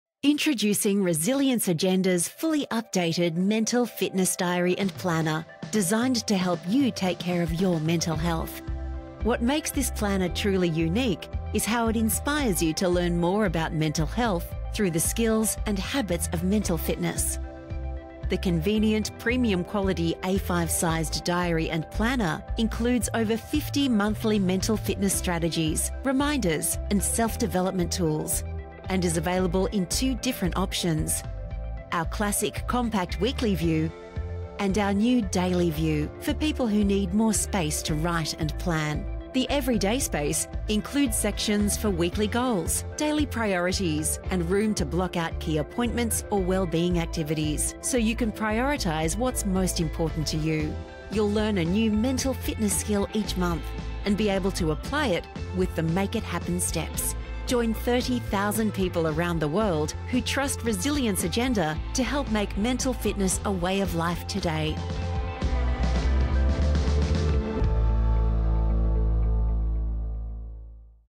2025 Mental Fitness Diary & Planner Features
Unlock a proactive approach to Mental Fitness with the 2025 Mental Fitness Diary & Planner.

Designed to empower your daily routines, this all-new and fully-updated Diary & Planner is packed with powerful tools and insights to help you make Mental Fitness a way of life.

This video dives into the features of our 192-page Weekly and 400-page Day-to-a-page formats, showing how each one supports you in organizing your life while focusing on wellbeing. Discover practical tools, monthly themes, and daily prompts aimed at enhancing resilience, focus, and calm through ten core areas, including Movement, Sleep, Mindset, and Connection.

Perfect as a personal tool or a thoughtful end-of-year gift, the 2025 Mental Fitness Diary & Planner is ideal for anyone looking to prioritize their mental health.